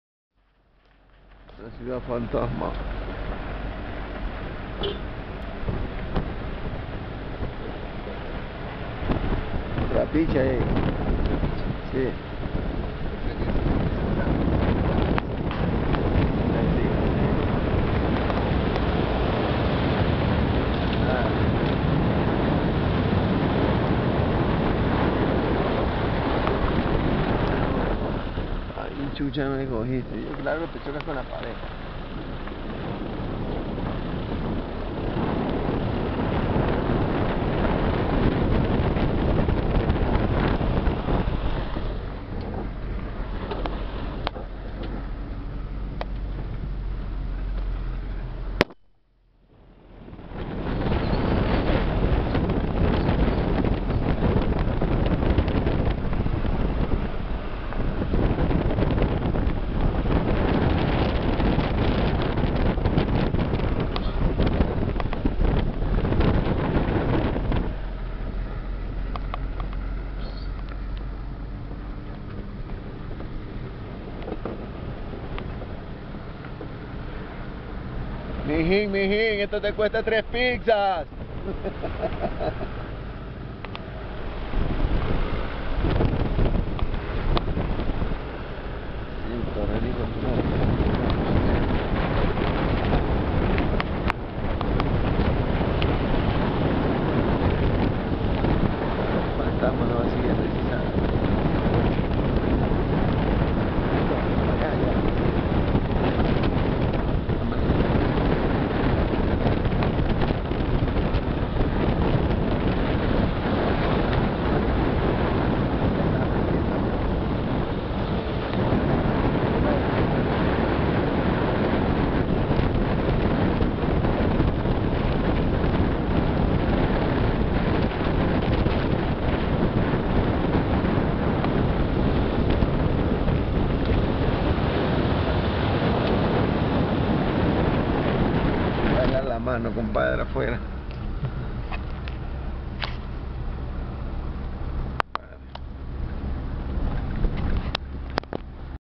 Manejando en Neblina Parte 2
Es la continuación del vídeo, claramente se aprecia el barranco, y las marcas en el camino anuncian futuros deslaves.